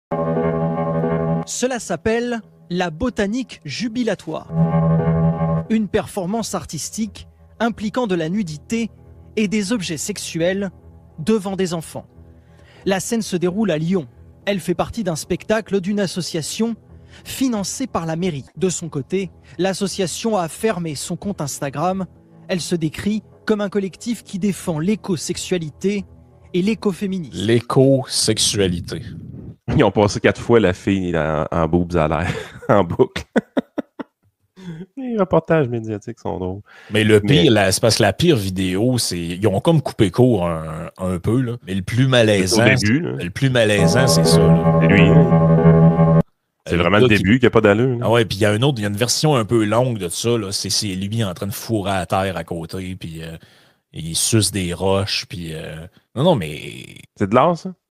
France : des « écosexuels » sont payés par l'État pour lécher des roches tout nus devant des enfants
Est-ce que c'est ça l'avenir du mouvement écolo et de la gauche occidentale?🤔

Extrait du podcast de Ian & Frank, disponible sur YouTube.